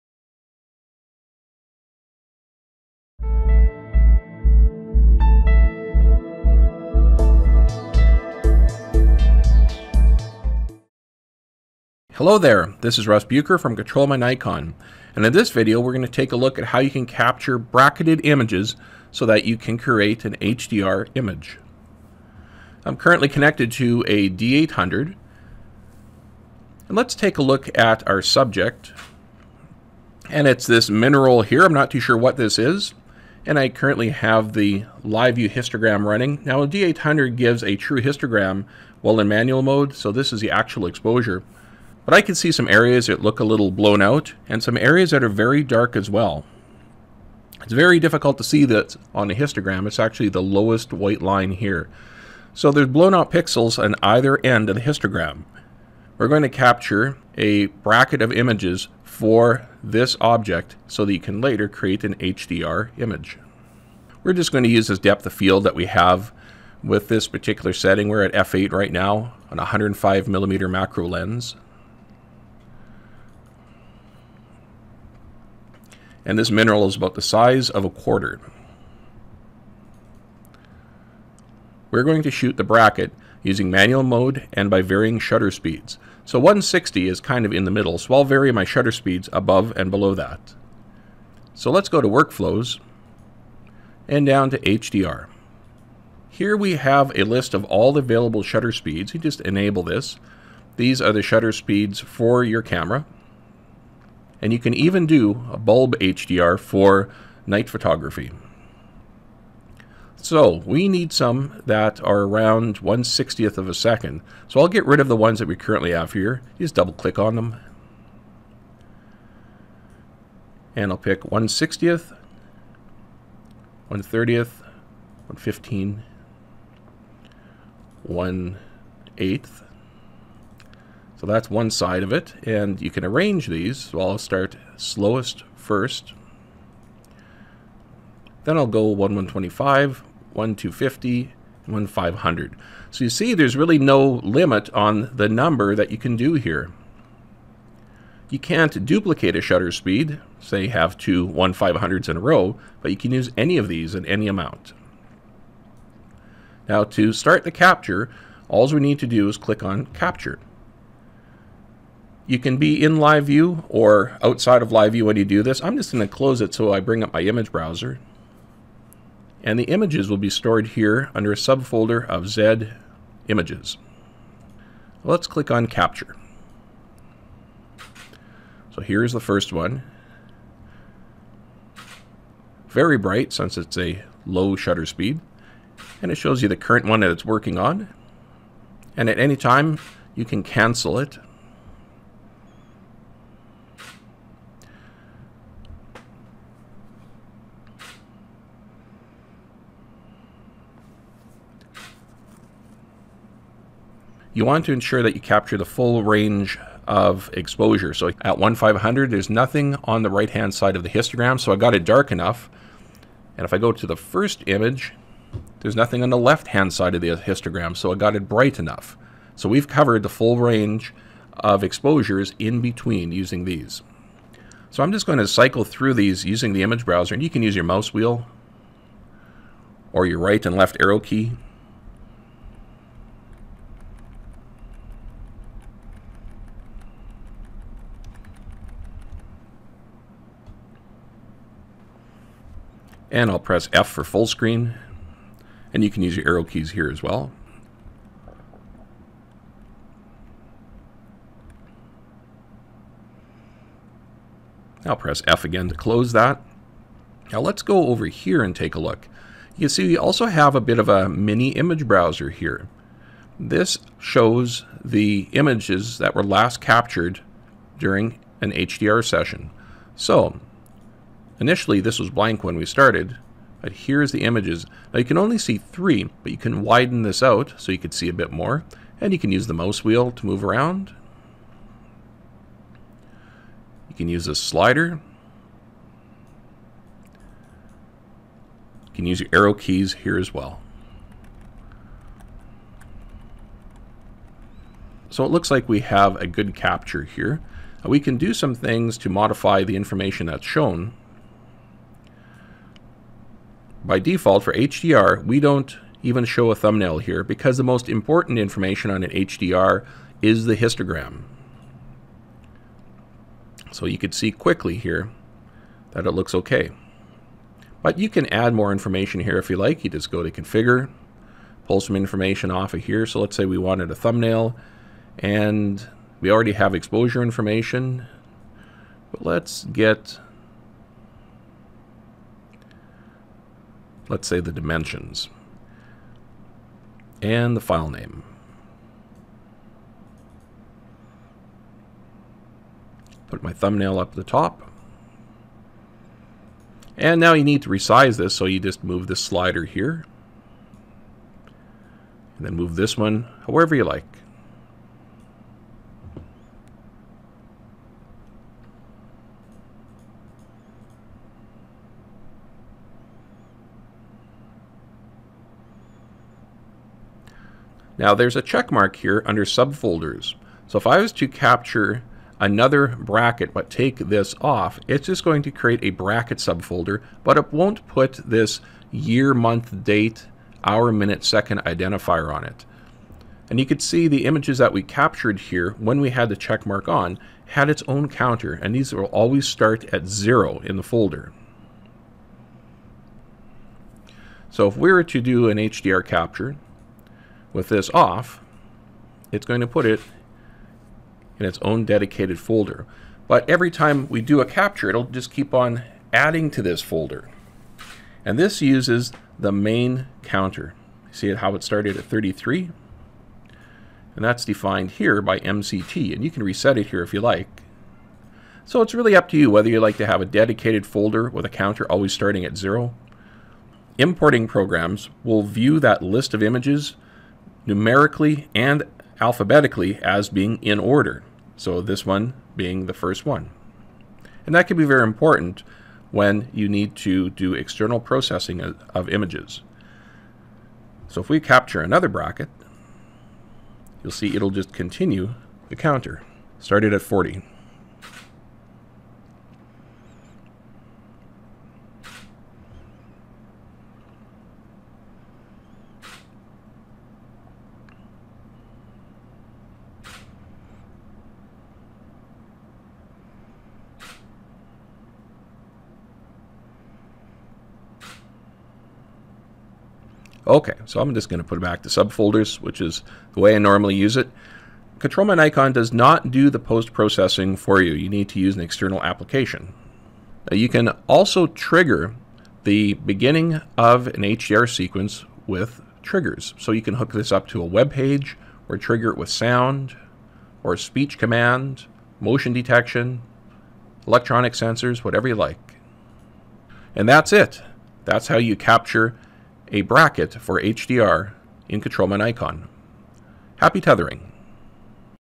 ControlMyNikon 5 - HDR Workflow
Shows how to capture the images needed to create a HDR image.